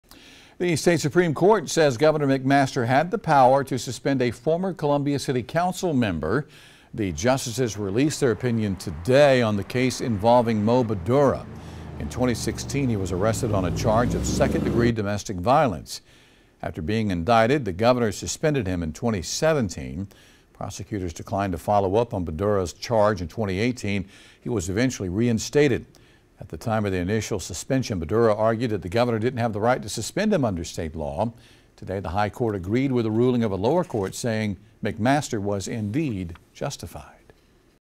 SC Supreme Court says governor had authority to suspend Columbia councilman
The S.C. Supreme Court said Gov. Henry McMaster acted within his authority when he suspended a former member of the Columbia City Council several years ago. More